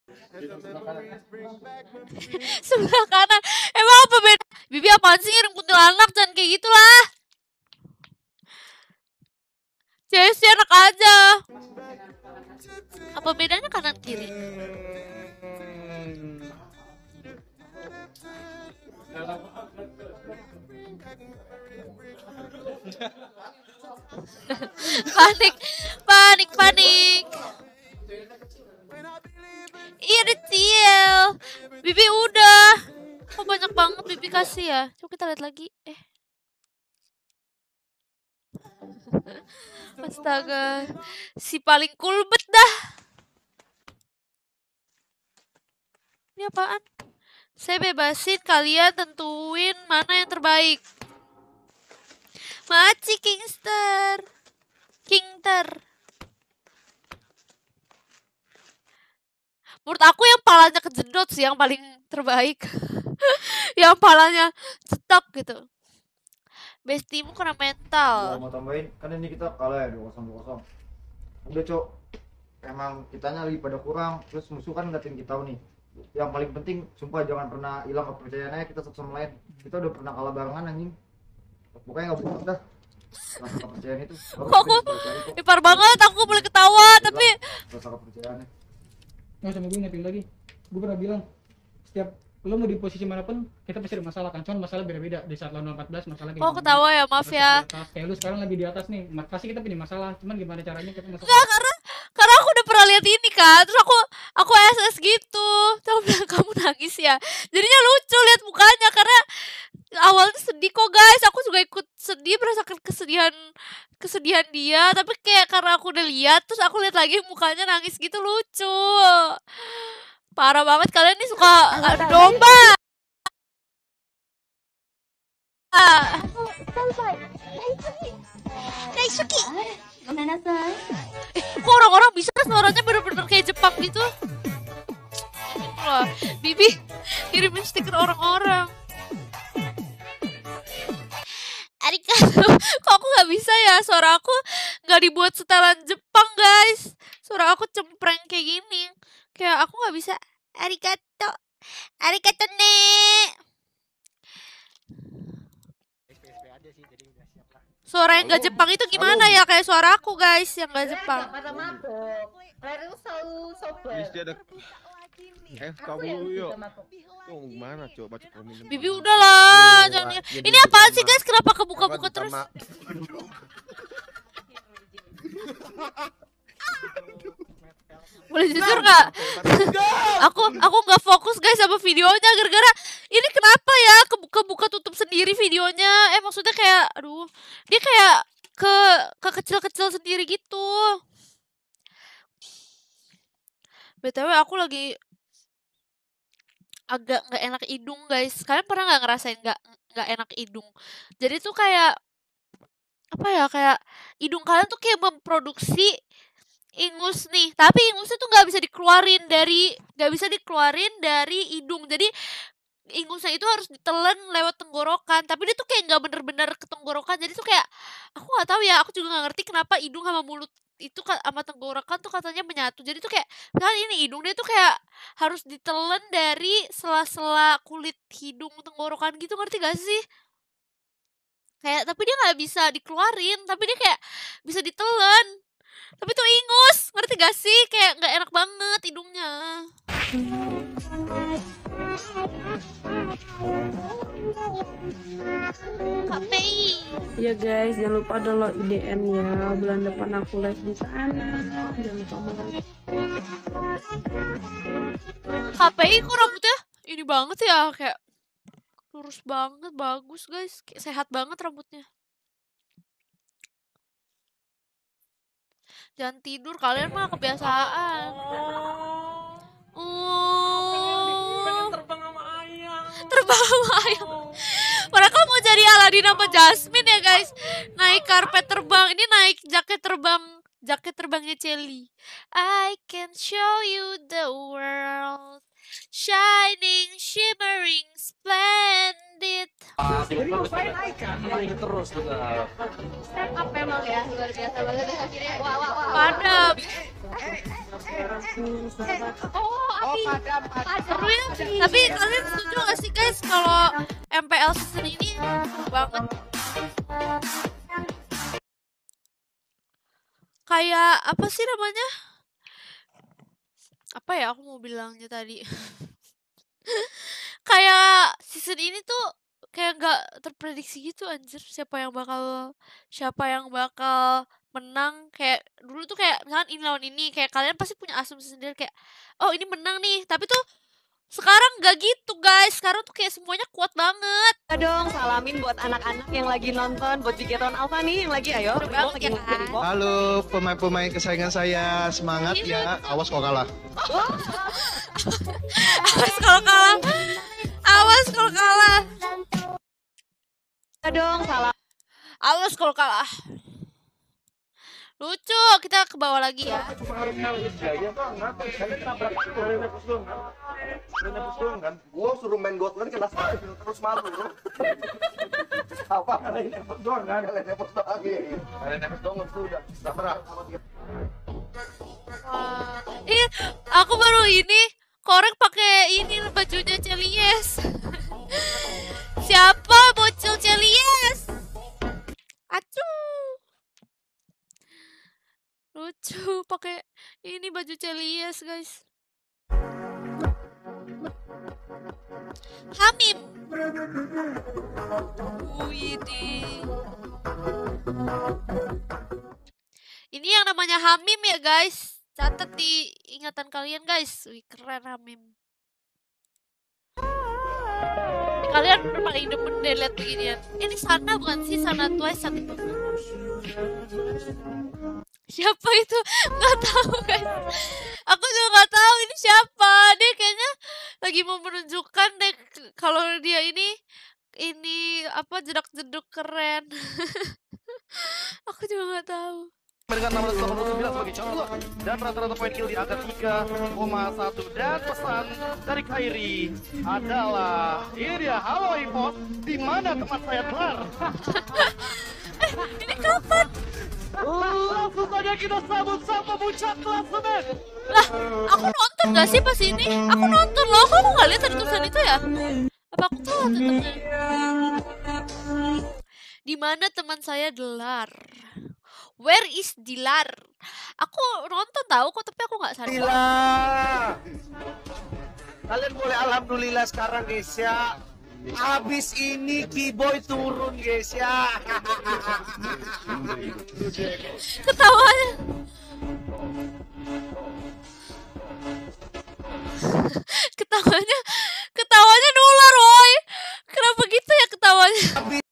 Reaksi CESI Liat KAYES Mau Terbang Sama Ayank - Lucunya CESI Ketika Liat Muka KABUKI Nangis
REAKSI CESI LIAT KAYES MAU TERBANG SAMA AYANK
LUCUNYA CESI KETIKA LIAT MUKA KABUKI NANGIS
REAKSI CESI LIAT REKT PAKAI BAJU CELIYES